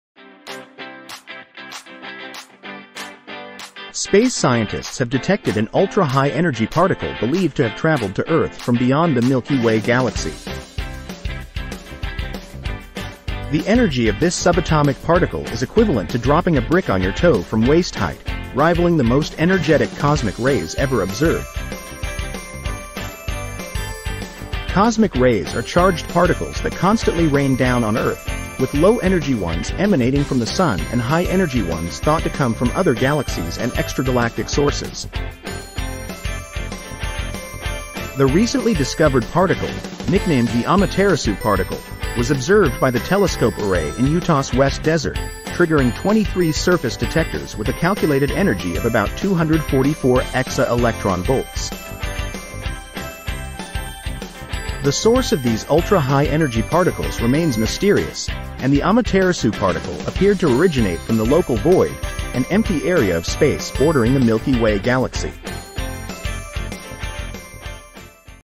Mysterious Cosmic Ray Observed in Utah
Space scientists have detected an ultra-high-energy particle believed to have traveled to Earth from beyond the Milky Way galaxy.
The energy of this subatomic particle is equivalent to dropping a brick on your toe from waist height, rivaling the most energetic cosmic rays ever observed.
Cosmic rays are charged particles that constantly rain down on Earth, with low-energy ones emanating from the sun and high-energy ones thought to come from other galaxies and extragalactic sources.
The recently discovered particle, nicknamed the Amaterasu particle, was observed by the Telescope Array in Utah's West Desert, triggering 23 surface detectors with a calculated energy of about 244 exa-electron volts.
The source of these ultra-high-energy particles remains mysterious, and the Amaterasu particle appeared to originate from the Local Void, an empty area of space bordering the Milky Way galaxy.